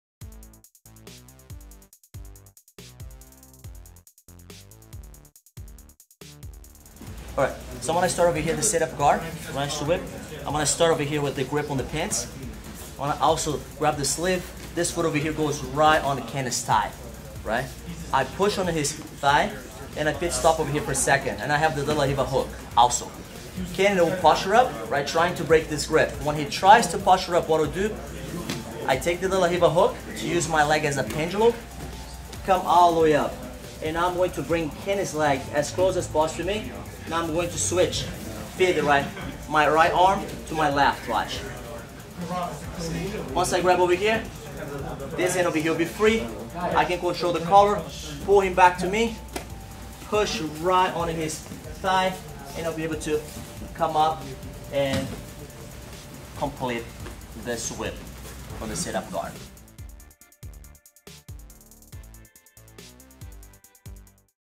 Cobrinha: a quick way to sweep from the sit up guard
Four times world champion and two times ADCC champion, Rubens Cobrinha is rightly considered one of the best competitors of all times. Here we find him teaching exclusively to GRACIEMAG.com a simple, quick and efficient sweep from the sit up guard. Play and learn.